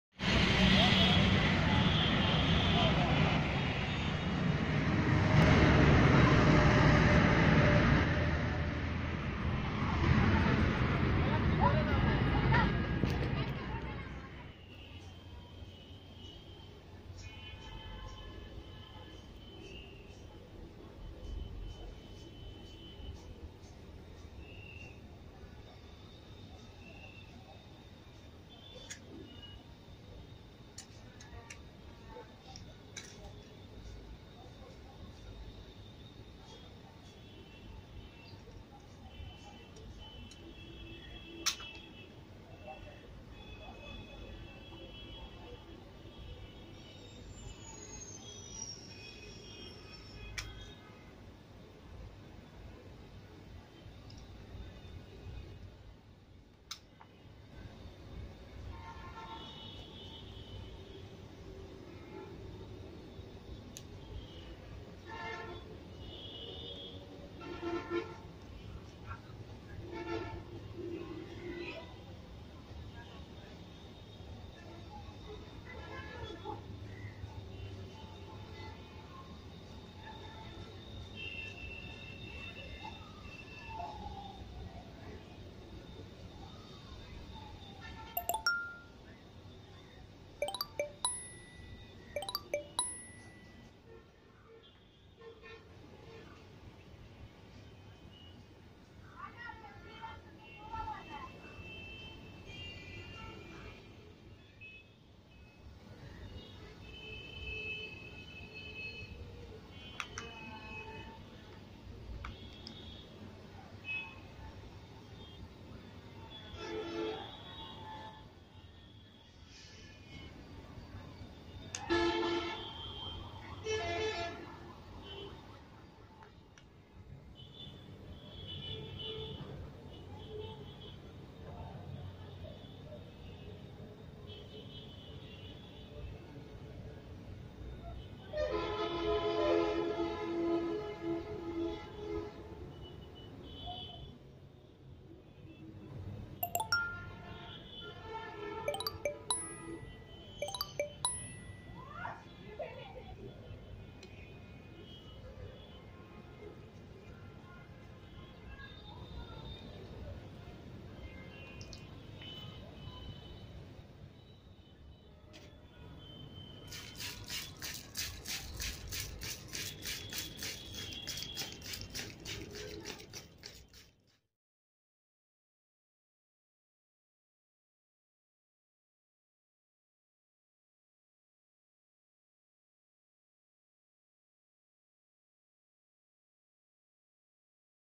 Grow Pinwheel or Crape Jasmine Cuttings in a totally new method | How to Make rooting Chandni plant.
The process is amazingly easy and everyone can do it . Only thing is that you can do the process in Monsoon months only.